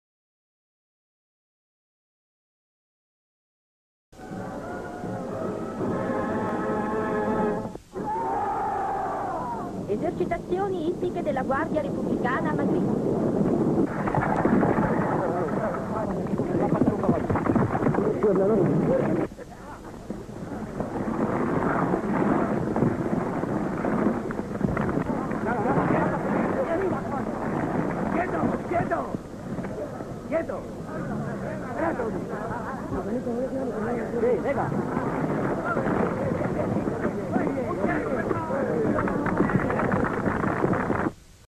Madrid: esercitazioni ippiche della Guardia Repubblicana
Descrizione sequenze:La Guardia Repubblicana spagnola compie esercitazioni ippiche ; passaggio di cavalli nel fuoco, salto con cavalli sotto un arco di uomini ; i cavalieri fanno percorrere ai cavalli una ripidissima discesa ;